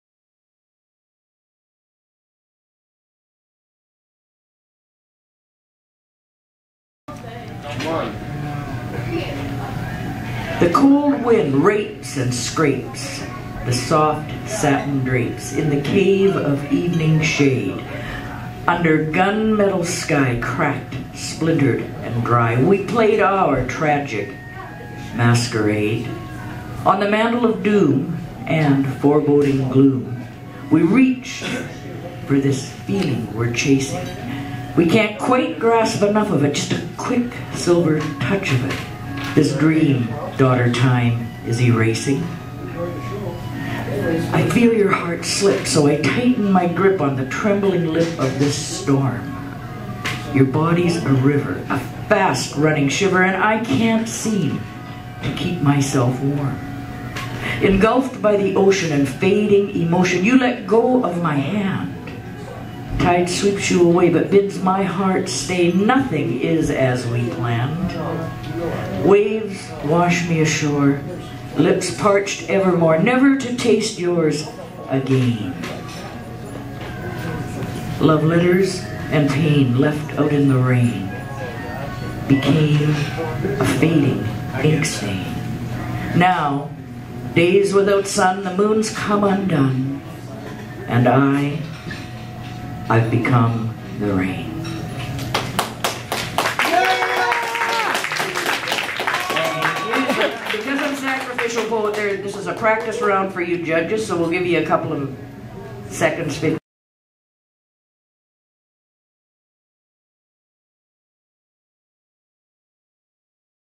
Slam Central - Candice James- October 27, 2011
Slam Central - Candice James- October 27, 2011 - Sacrificial Poet, Slam Central, Heritage Grill, New Westminster, Thurs nights

Ink Stain in the Rain Copyright Candice James